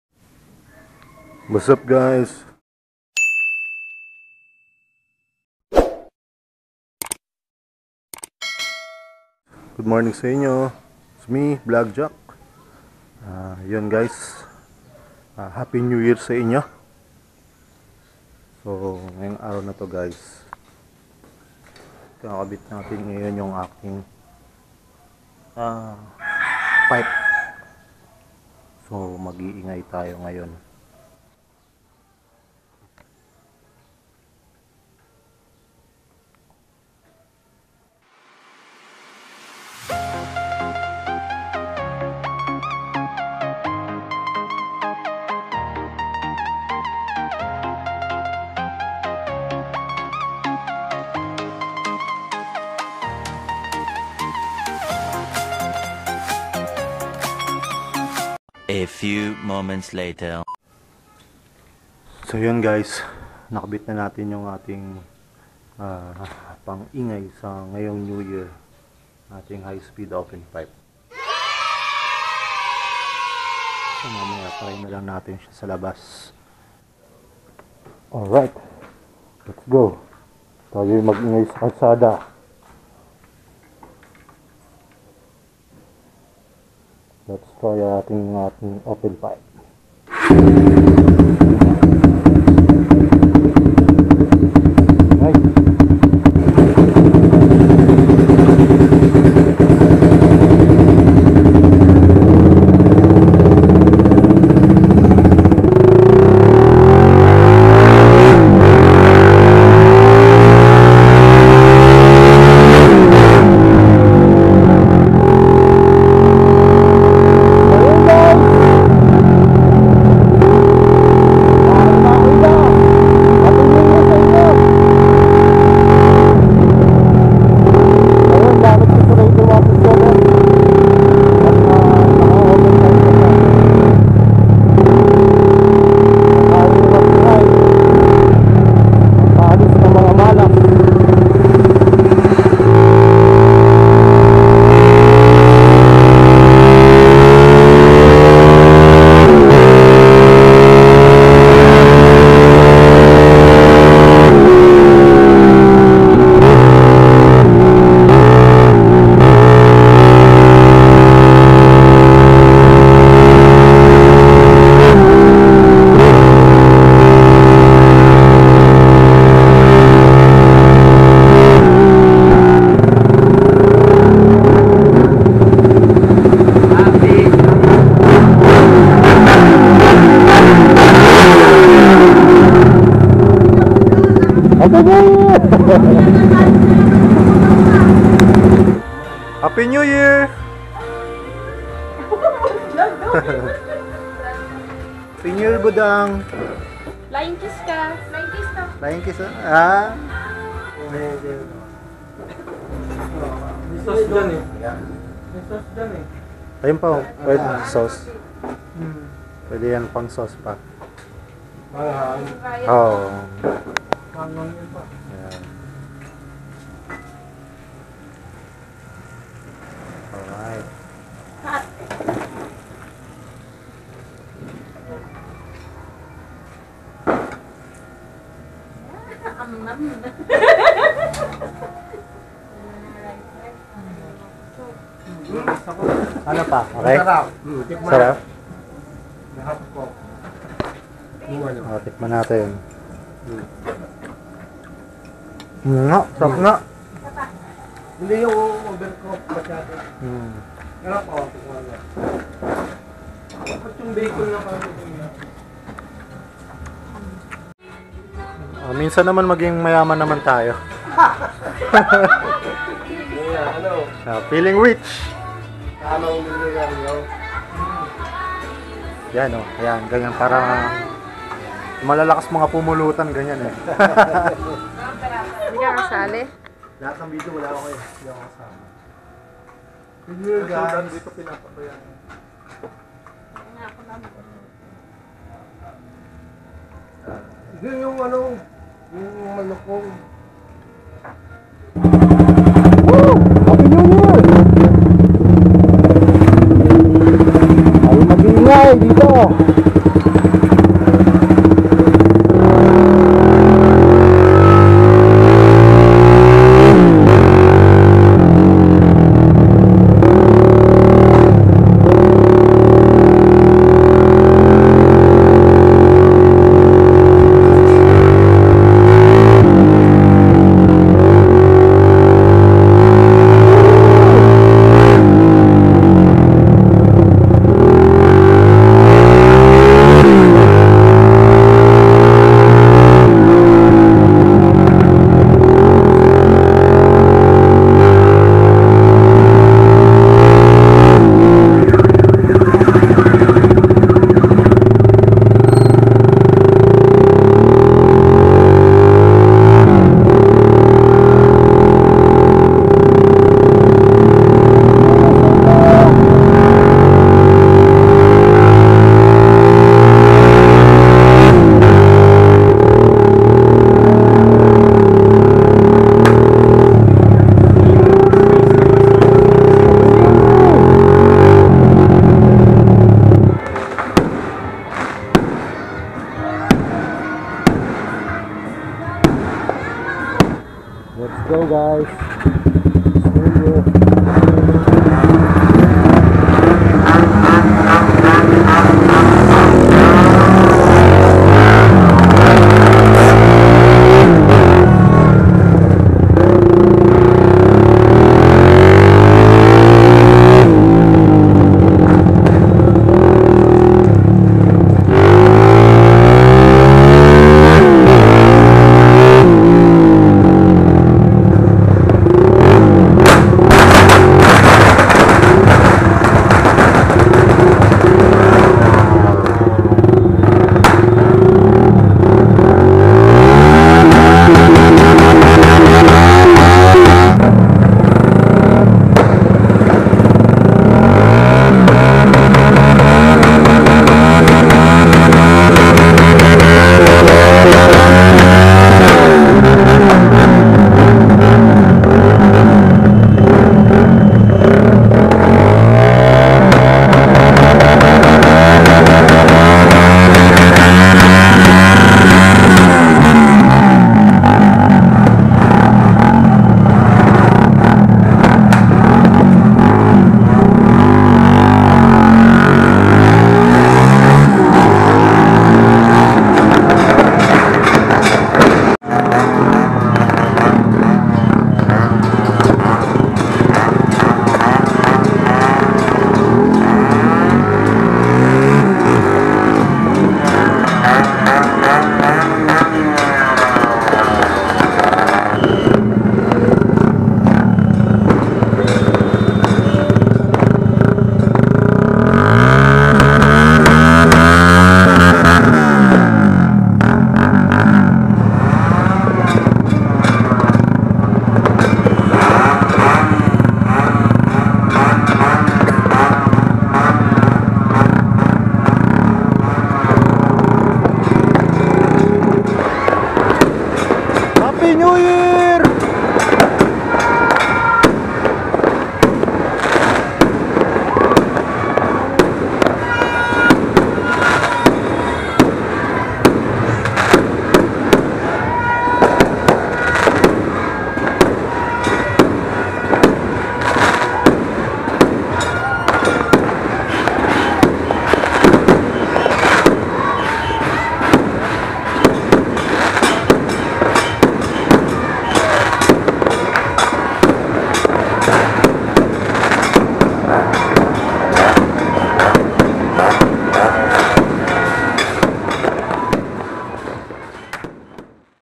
HIGH SPEED OPEN PIPE | RAIDER 150 | HAPPY NEW YEAR WELCOME 2020 TAYU MAG-INGAY SA MALIGAYA PARK SUBD
Wazzup guyz HAPPY NEW YEAR SAINYU..  tayu'y mag-ingay at salubingin ang taong 2020